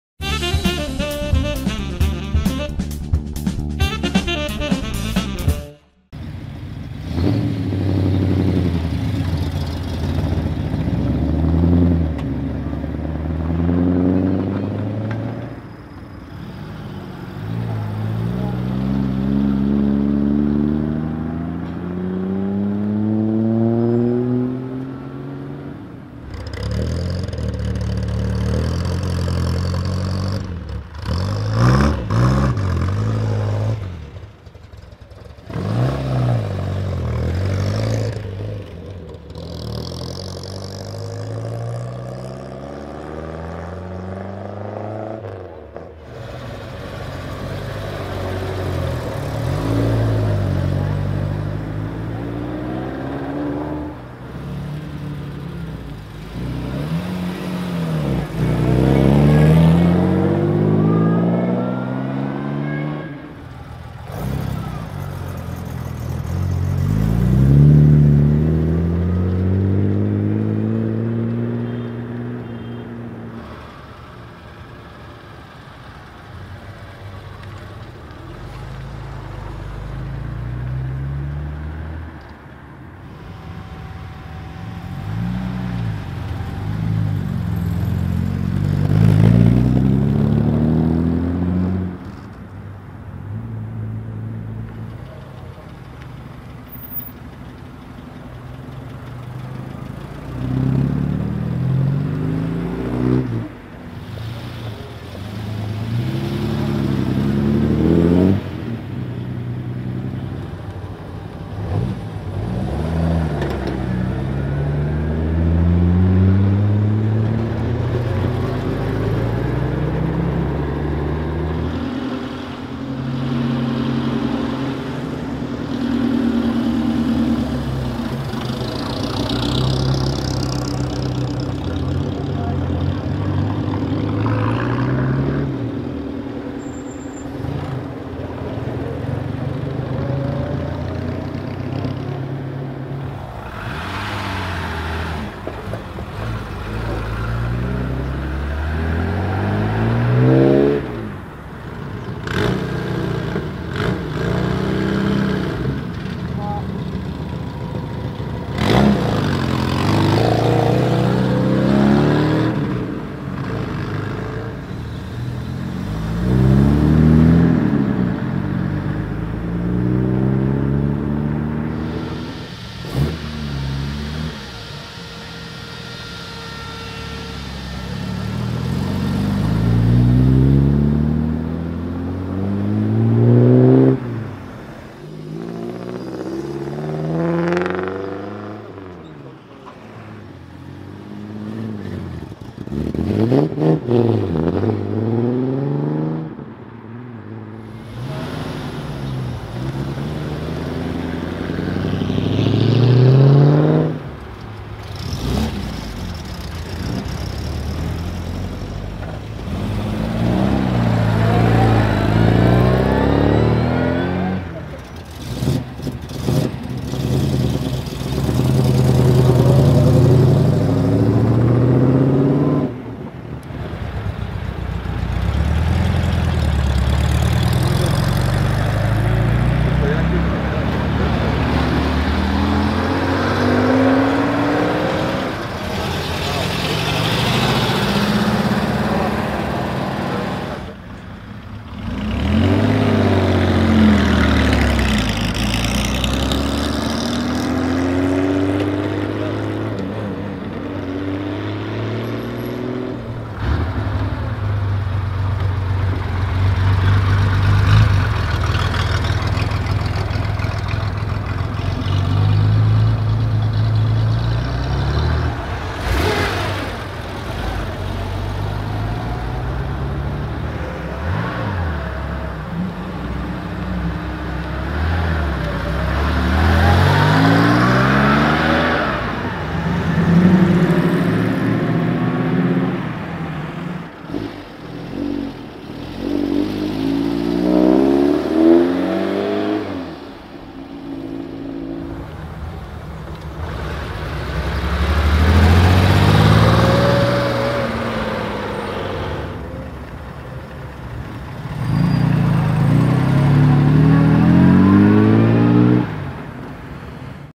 +30 Clásicos acelerando | Hero CUP London to Lisboa 2017 (DB5, E-Type, Carrera RSR, 356, etc)
Vídeo en 1080p Full HD por Cocheschannel98. La tarde del día 27 de Abril de 2017 llegaron al fabuloso Parador de San Marcos los participantes del London to Lisboa 2017. La mañana del 28 de Abril empezaron a salir del parador, y ahí estábamos nosotros para filmarlo todo. Un gran número de clásicos exclusivos participaban en el evento. En el vídeo podréis ver coches tan raros como un Aston Martin DB2, un Porsche Carrera RSR, un Aston Martin DB5, un Jaguar E-Type 4.2 o un Porsche 356. Disfrutad del vídeo.